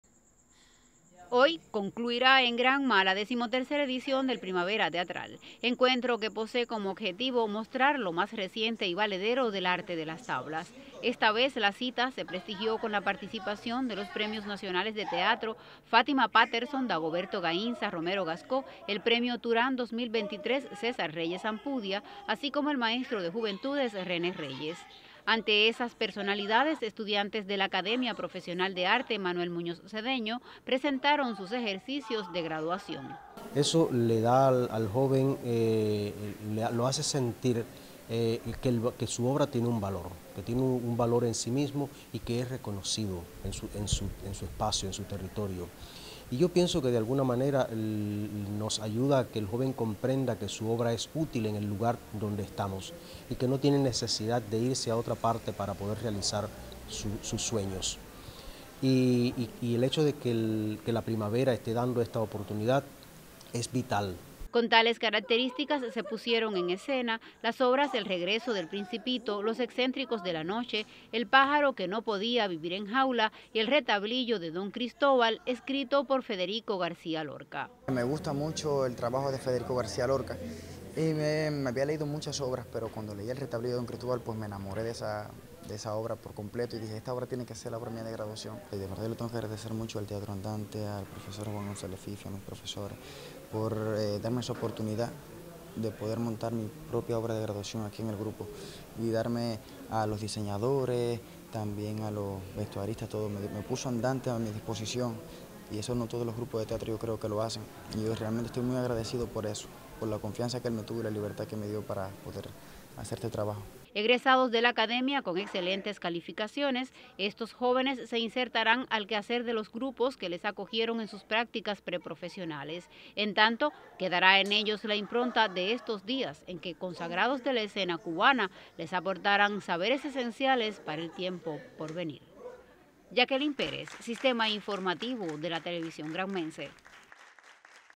Se integran estudiantes de actuación a espacios del Primavera Teatral
El Primavera teatral cierra sus puertas luego de cuatro jornadas de celebraciones. En esta oportunidad el programa de la cita incluyó como novedad visitas a comunidades suburbanas  y espacios interactivos con estudiantes de actuación.